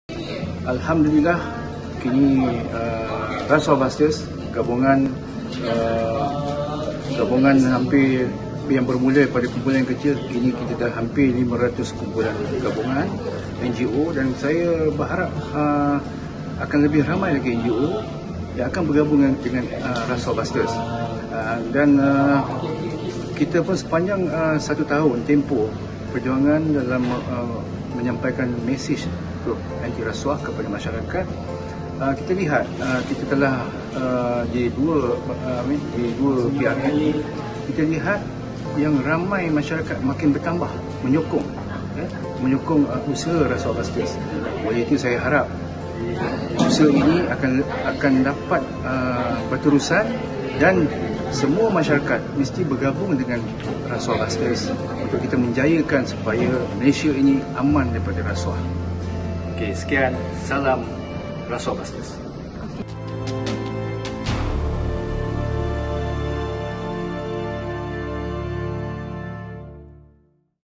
Salam RasuahBusters!
"Makin ramai masyarakat menyokong usaha RasuahBusters." - Pengerusi Yayasan Keprihatinan Komuniti Malaysia (MCCF), Datuk Seri Halim Ishak